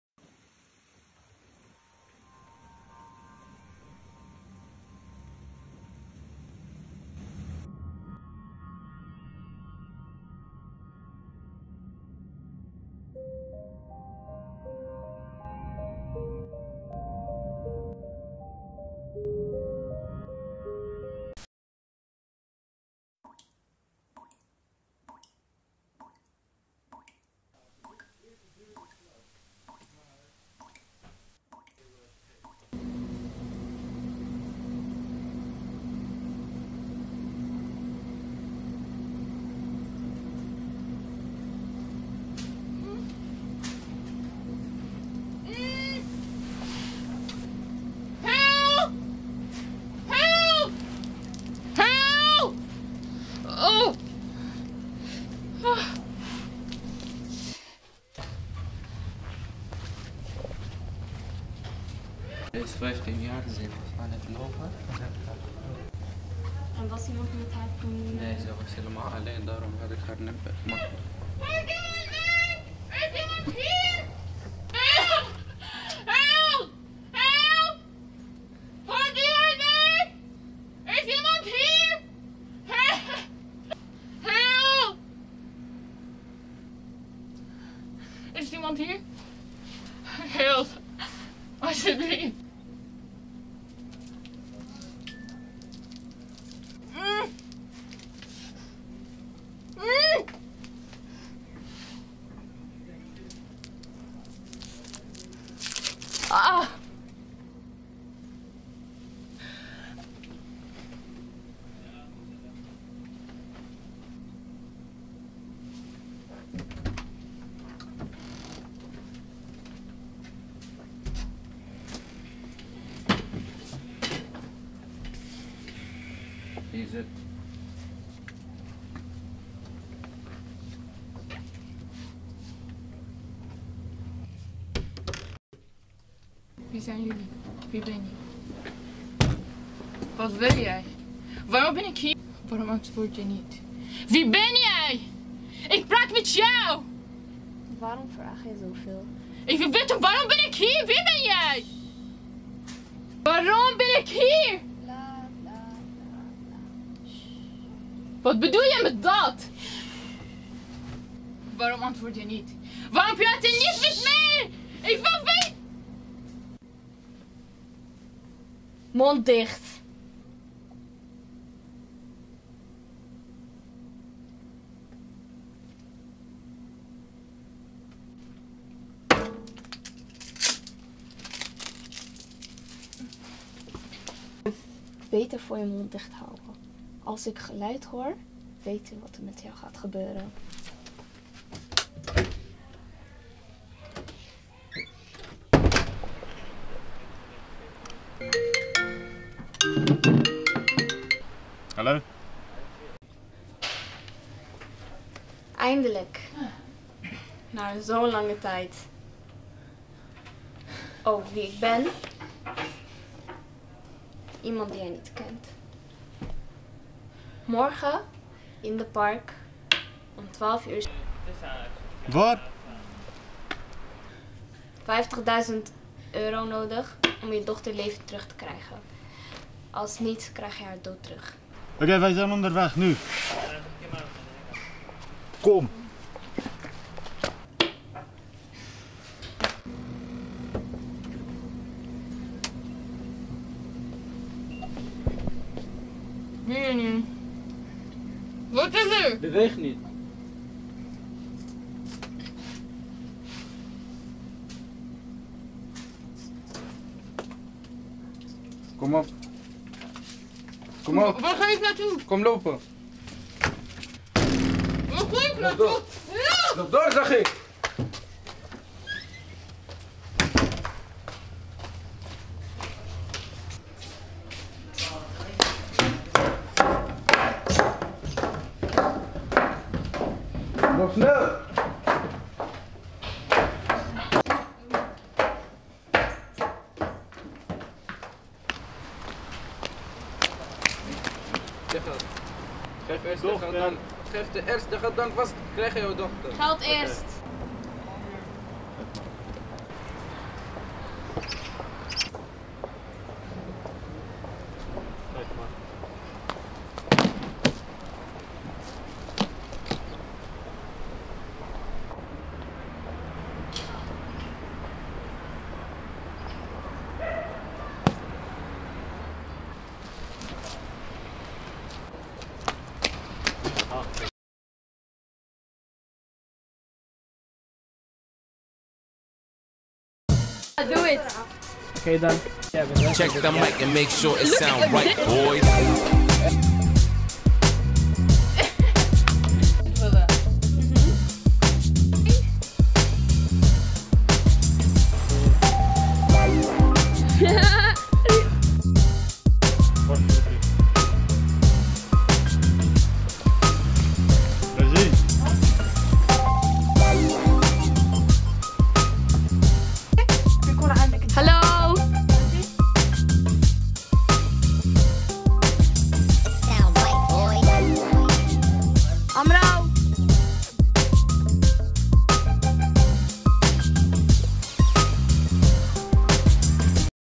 Ithaka Filmt 2021 - Big ass mafia gang
Film gemaakt door jongeren van de Ithaka ISK in Utrecht in het kader van het het project Ithaka Filmt. Meer informatie over het project